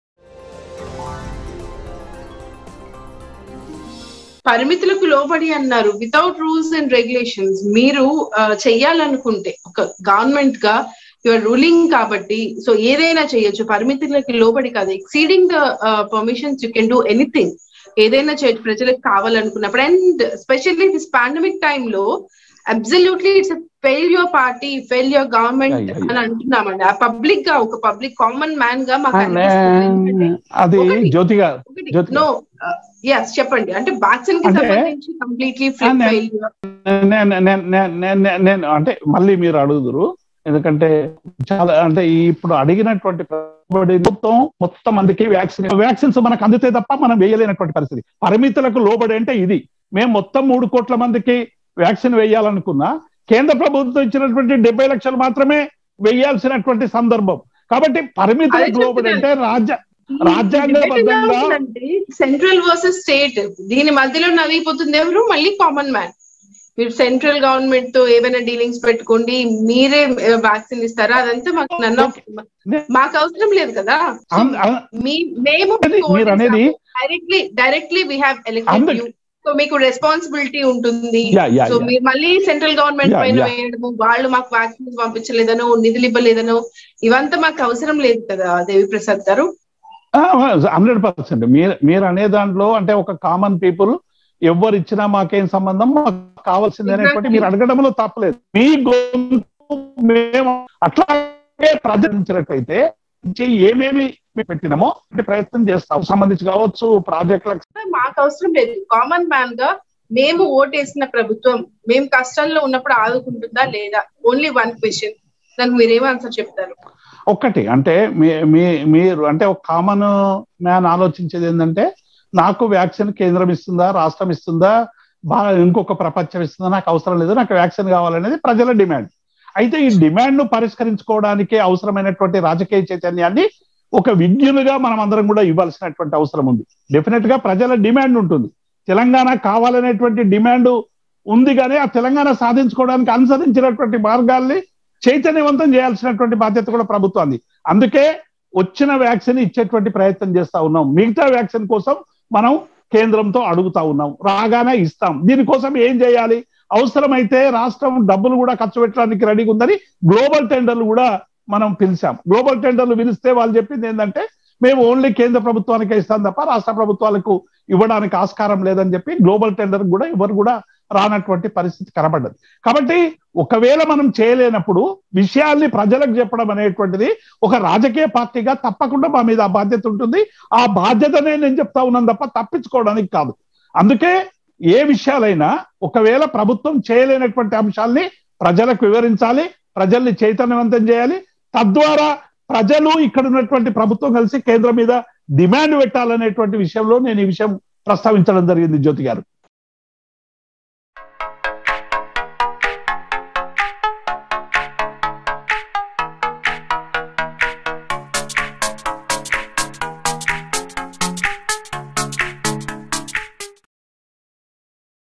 ప్రజలు కష్టంలో ఉన్నప్పుడు ప్రభుత్వం ఎందుకు ఆదుకోవట్లేదు? Devi Prasad Rao about TRS Government & Modi
Discussion on Telangana Politics | Etela Rajender Vs KCR | Huzurabad By-Election || S Cube TV

Watch here exclusive discussion with Devi Prasad Rao - TRS Leader and TSBCL Chairman by Journalist Gattamaneni Makarajyothi.